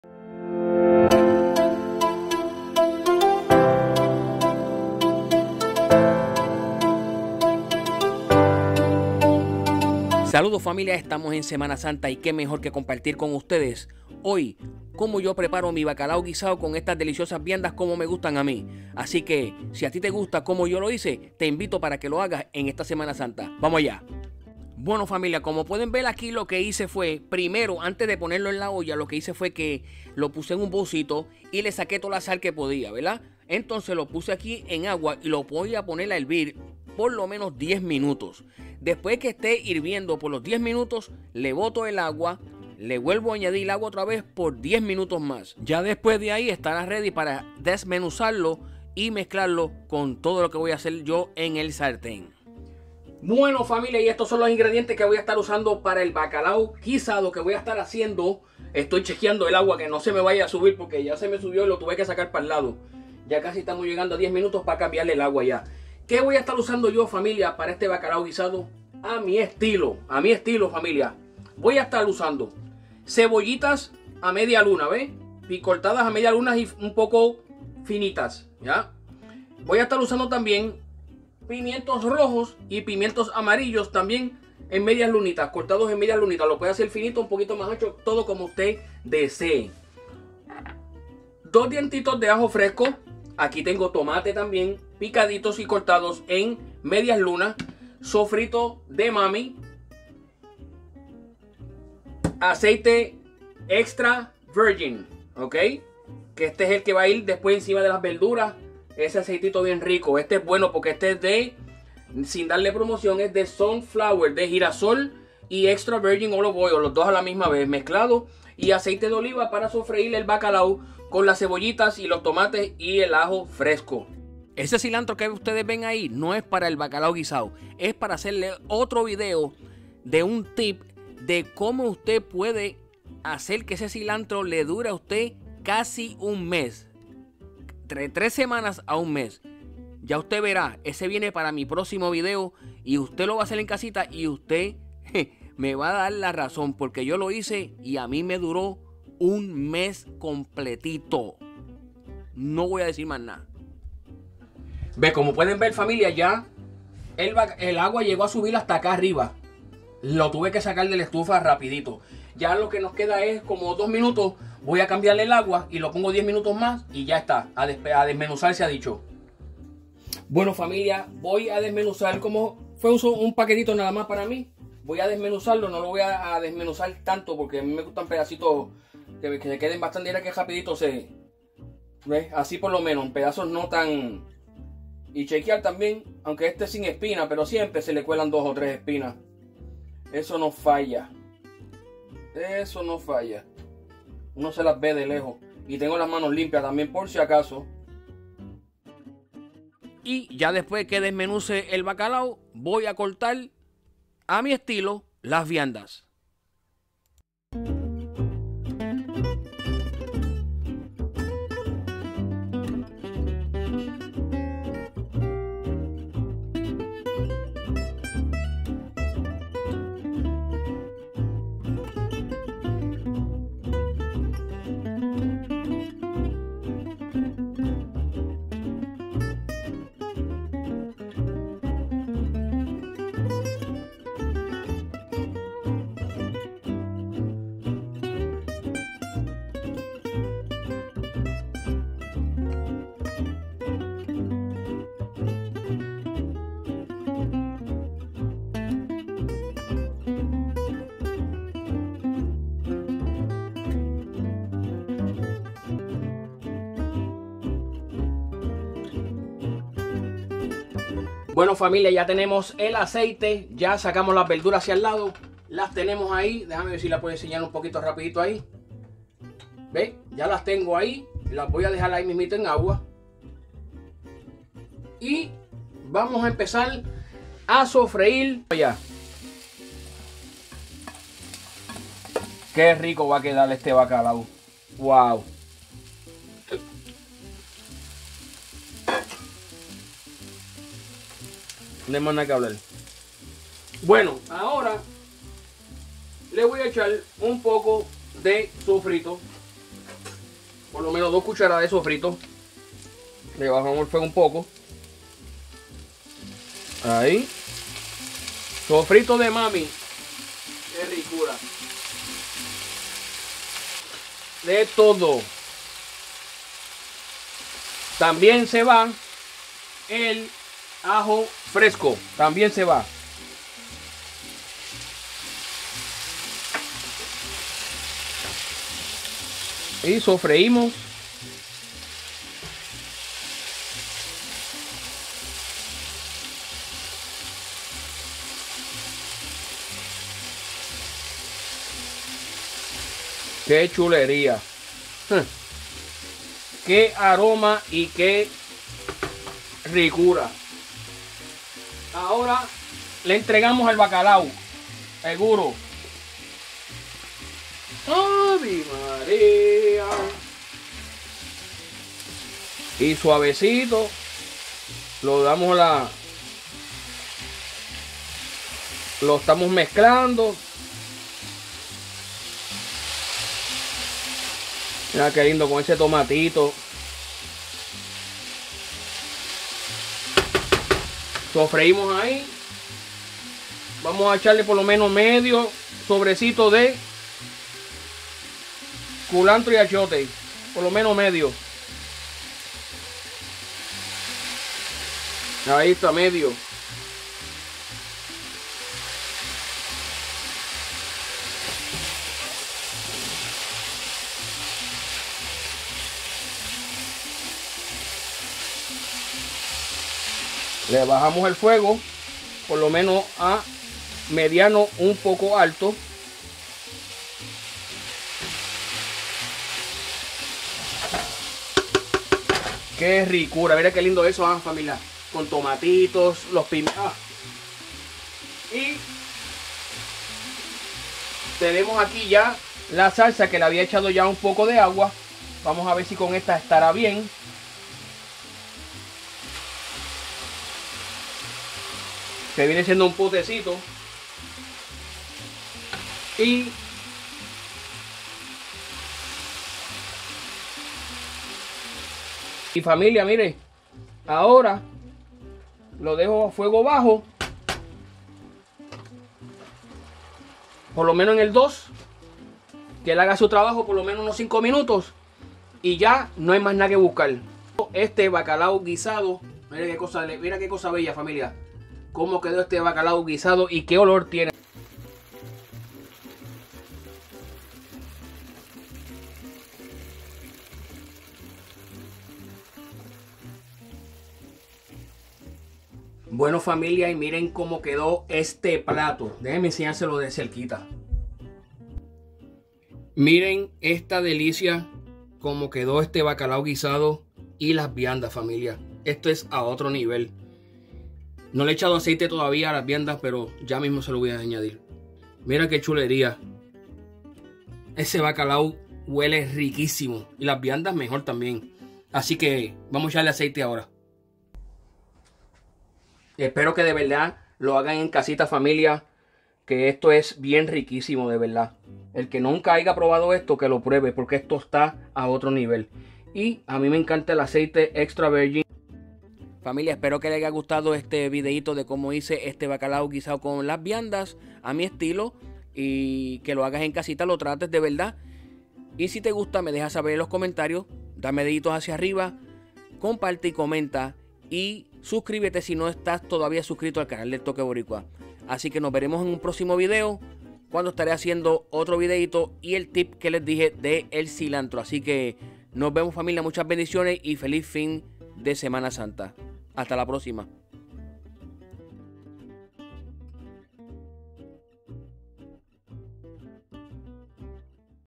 VIANDAS CON BACALAO GUISADO, A MI ESTILO
SALUDOS FAMILIA ESPERO LES GUSTE COMO LES COMPARTO ESTOS DELICIOSAS VIANDAS CON BACALAO GUISADO A MI ESTILO Y ESPERO LES GUSTE.

ingredientes:
BOLSA DE 12 OZ DE BACALAO
1 CEBOLLA MEDIANA
1 PIMIENTO AMARILLO
1 PIMIENTO ROJO
1 SOBRECITO DE CULANTRO Y ACHIOTE
2 CDAS DE SOFRITO
ACEITE DE OLIVA
ACEITE XTRA VIRGEN
2 DIENTES DE AJO FRESCO
1 LATA DE SALSA DE TOMATE
2 TOMATES (ROMA)

DE VERDURAS O VIANDAS USE
 BATATA,MALANGA ETC PUEDEN USAR CUALKIERA DE SU PREFERENCIA.
CUALKIERA PREGUNTA ME LA HACEN EN LOS COMENTARIOS👍🇵🇷

LA MUSICA QUE ESCUCHAN ES DE

AGARRADERAS PARA LA COCINA CON LOGO DE PUERTO RICO
POT HOLDERS SET(PUERTO RICO)
SALSA LIZANO (COSTA RICAN SAUCE)